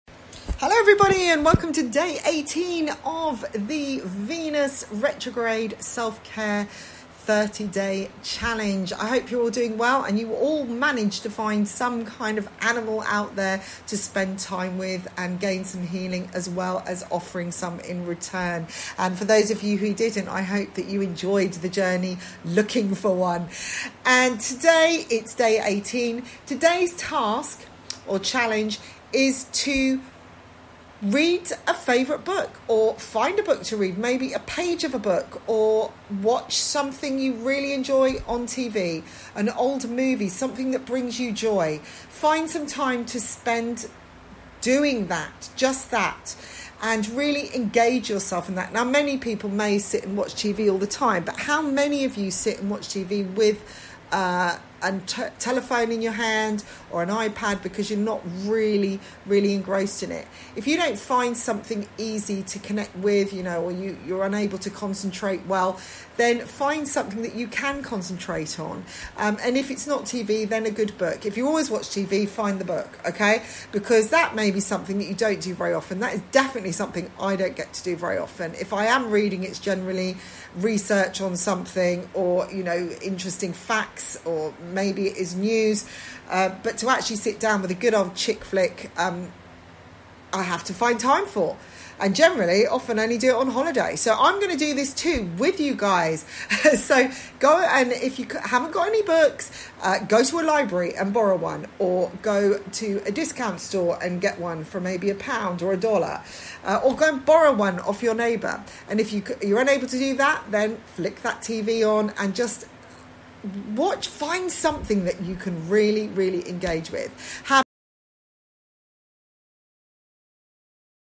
Venus Retrograde 30 day Self - Care Challenge day 18
Welcome to DAY 18 of the VENUS RETROGRADE SELF-CARE challenge. Re-balance the emotions by re-claiming your HEART. Give a LITTLE BACK to YOURSELF each DAY and create a NEW POSITIVE PATTERN in your REGULAR ROUTINE.  TAKING TIME to COMMIT to these SIMPLE and EASY DAILY tasks will KICKSTART you into a MORE FULFILLING FUTURE.

COPY AND PASTE THE LINK BELOW and Share Progress and Connect with other HEART WARRIORS!